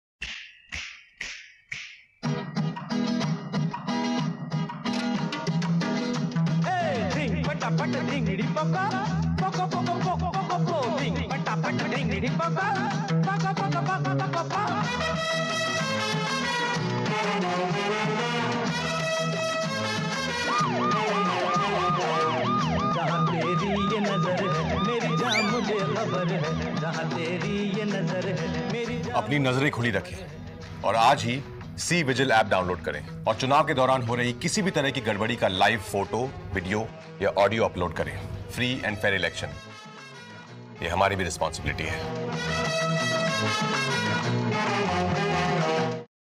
Watch | Use the cVIGIL app 📲 and report violations of the Model Code of Conduct.
🤳 Snap, Report, Empower! ✨🙌

Use the cVIGIL app 📲 and report violations of the Model Code of Conduct.